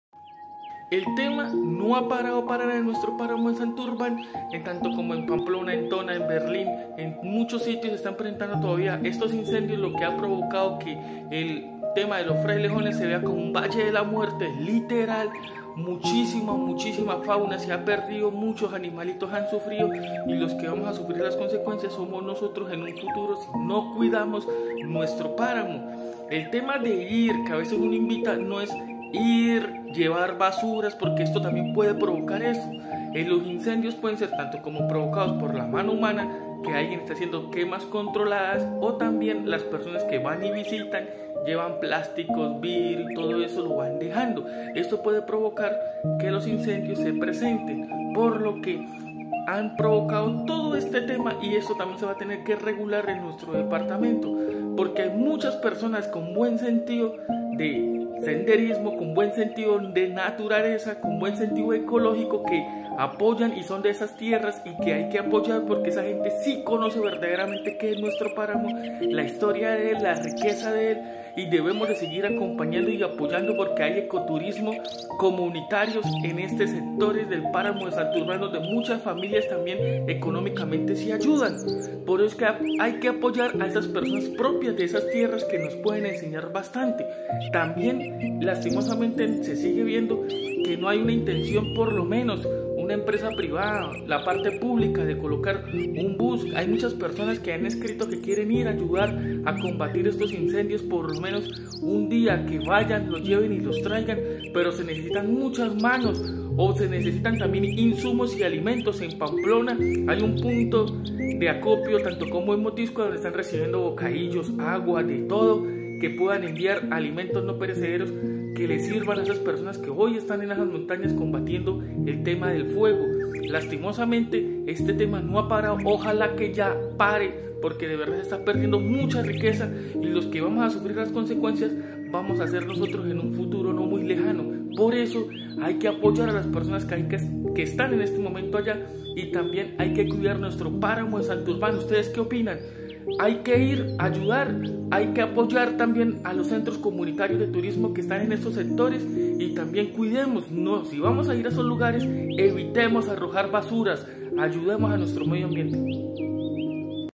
urgente El fuego sigue pero nuestros campesinos
urgente El fuego sigue pero nuestros campesinos, ejercito, bomberos, defensa civil, policia y personas que han llegado estan luchando por salvar a nuestro ·paramodesantaruban con los IncendiosForestales que han vuento en valles de frailejones de la muerte

no podemos abandonar a todas estas personas que hoy estan luchando por salvar a nuestro paramo por eso lo invito que em pamplona y en mutiscua hay puntos de acopio para que lleven alimento no perecederos y bocadillo y agua para ayudar a todas estas personas, entendamos que los rios zulia pamplonita y arauca nacen en este paramo y debemos de cuidarlo y protegerlo.